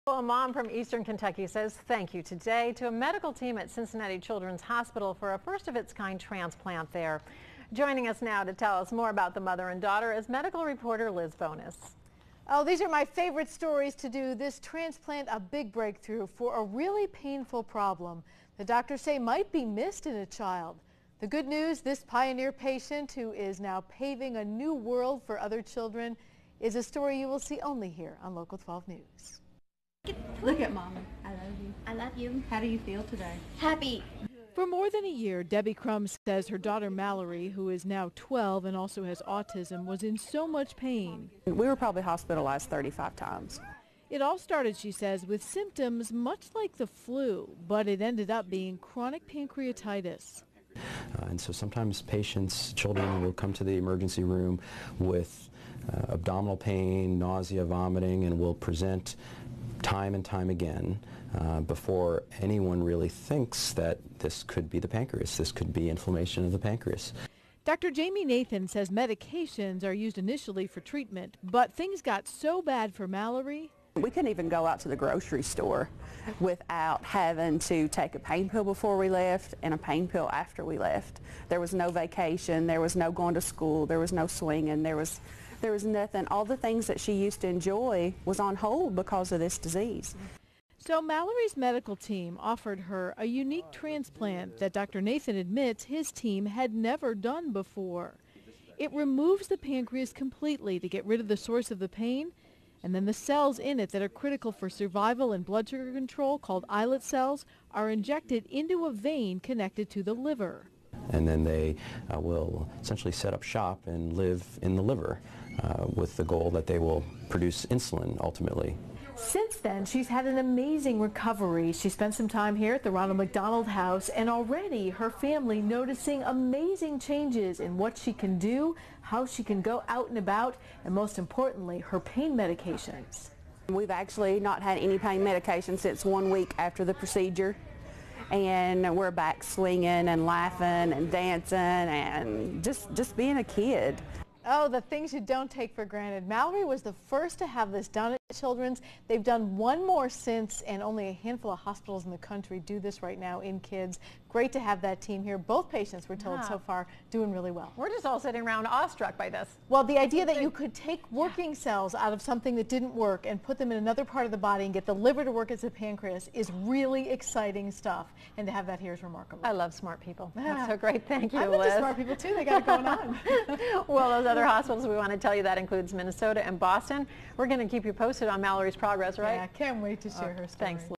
Islet Cell Transplant performed at Children's Hospital
CINCINNATI (Liz Bonis) -- A mom from eastern Kentucky said thank you Tuesday, July 7, to a medical team at Cincinnati Childrens Hospital for a first of it's kind transplant there.      The transplant was a big breakthrough for a painful problem that doctors said might be missed in a child. The good news was the pioneer patient is now paving a new world at Childrens for other kids. For more than a year Debbie Crum said her daughter, Mallory, who is now 12 and also has autism, was in so much pain. Debbie said, We were probably hospitalized 35 times. It all started she said with symptoms much like the flu. But it ended up being chronic pancreatitis. Dr. Jaime Nathan, a pediatric transplant surgeon, said, Sometimes patients children will come to the emergency room with abdominal pain, nausea, vomiting and present time and time again before anyone really thinks that this could be the pancreas. This could be inflammation of the pancreas. Doctor Nathan said medications were initially used for treatment but things got so bad for Mallory the family couldn't even go to the grocery store without having to take a pain pill before they left. There was no vacation, there was no going to school, there was no swinging; there was nothing. All the things she used to enjoy were on hold because of the disease. So Mallory's medical team offered her the chance to have a unique transplant. Doctor Nathan admitted his team had never done a surgery like that before. It removes the pancreas completely to remove the pain and then transfers cells in it critical for survival and blood sugar control, called islet cells, into a vein connected to the liver. And then they will eventually set up shop and live in the liver with the goal that they will produce insulin ultimately, said Dr. Nathan. Since then Mallory's had an amazing recovery. She spent some time at the Ronald McDonald House and already her family noticed what she can do; how she can go out and about and more importantly her pain medication. We've actually not had any pain medicine since one week after the procedure and we're back swinging and laughing and dancing and just being a kid, said Debbie. Mallory was the first to have this done at Children's. Doctors have done one more since. Only a handful of hospitals in the country do this in children and its nice to have that team in the Tri-State. Both patients are doing well. Other hospitals that perform the procedure include Minnesota and Boston. Follow Liz Bonis on Twitter @lbonis1, and LIKE her on Facebook.